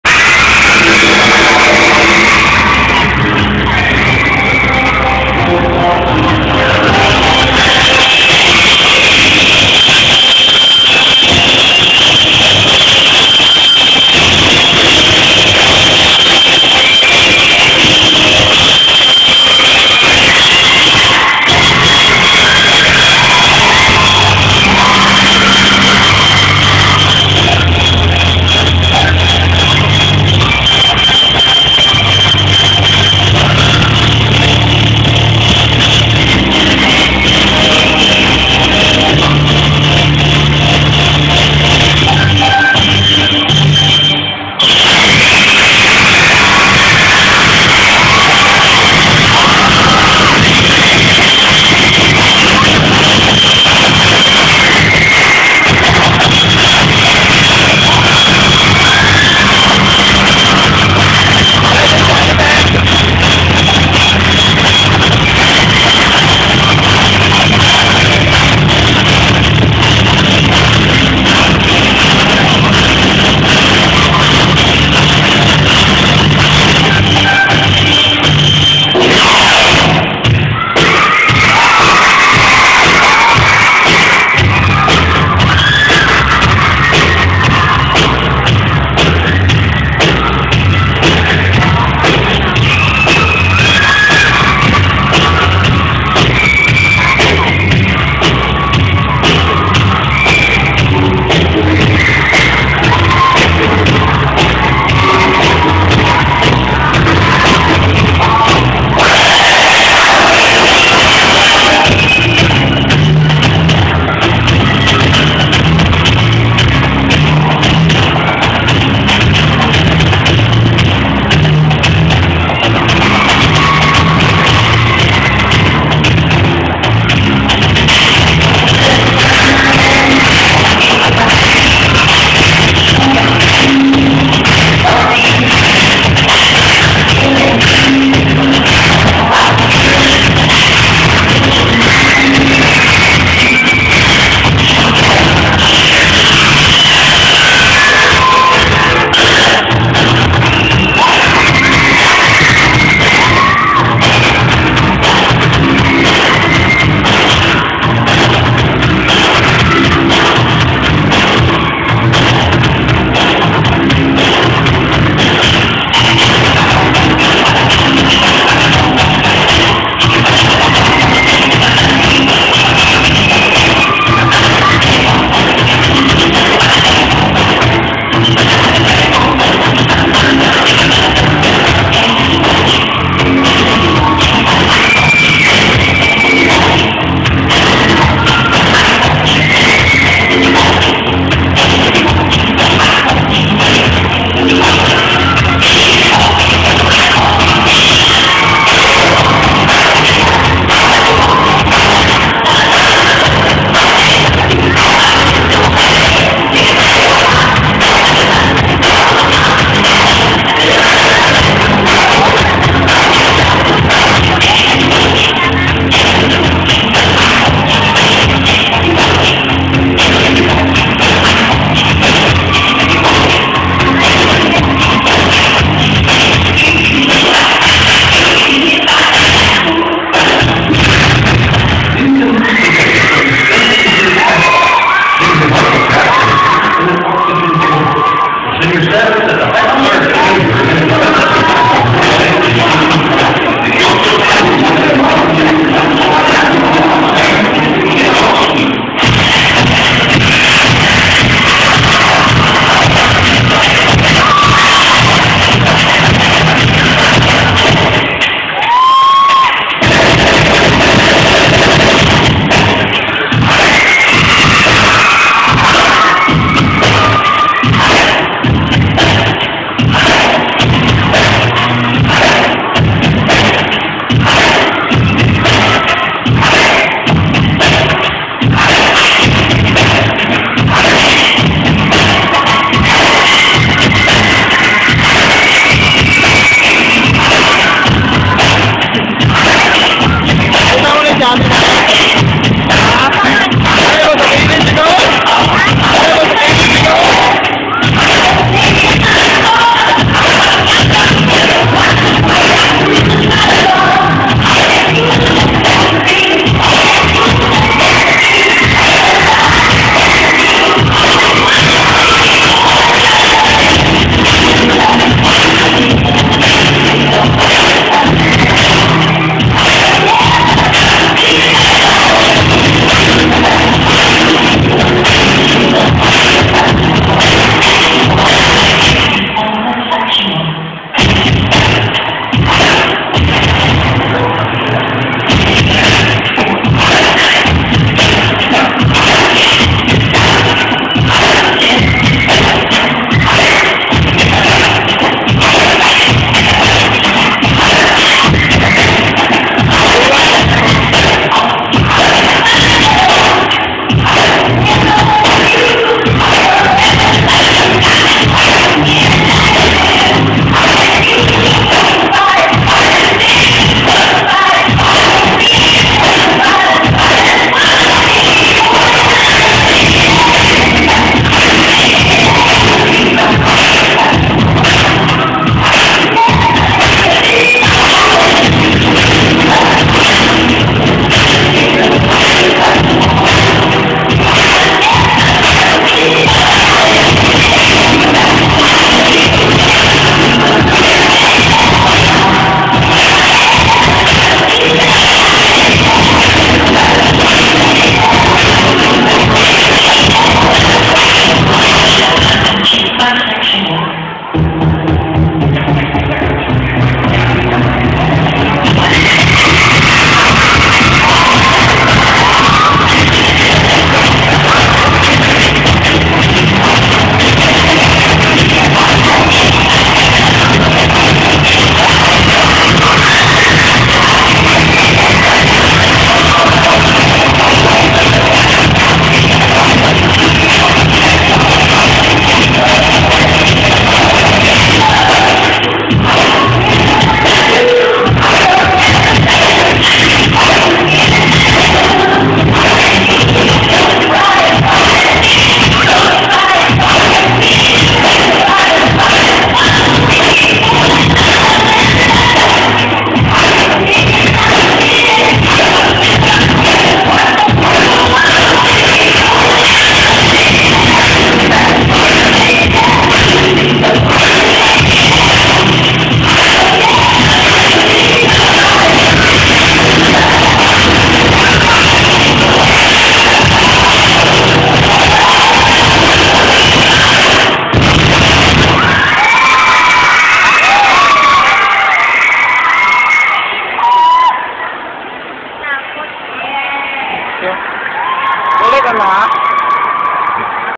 ALC Happy Hearts '10 - Gr. 5 Jackson
Panks', our trainor since Gr. 3, version of the dances of the One and Only King of Pop, Michael Jackson!